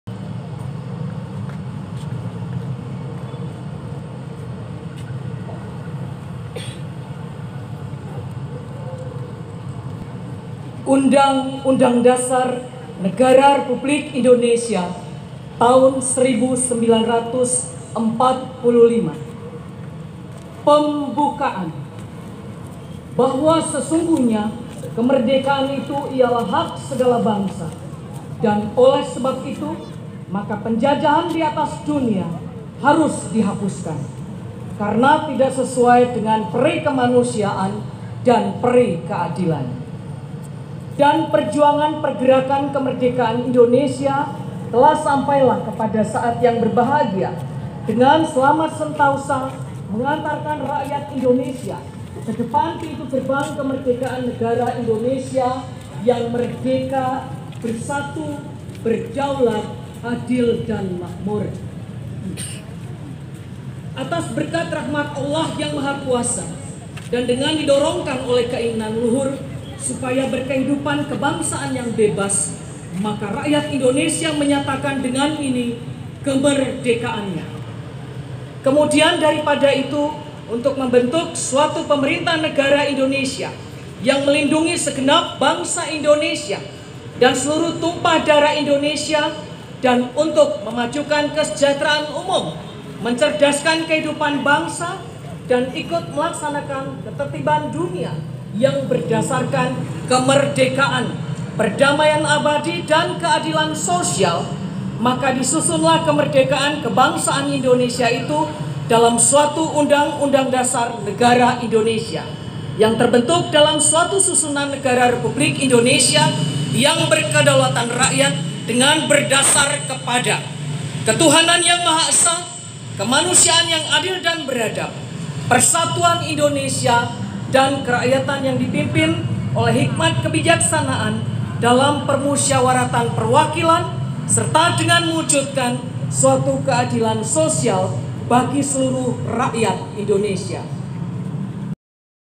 terharu pembacaan undang undang dasar 45 upacara bendera Hari Kemerdekaan Karawang karangpawitan
undang undang dasar 45 upacara bendera Kemerdekaan Republik Indonesia ke-77 Karawang karangpawitan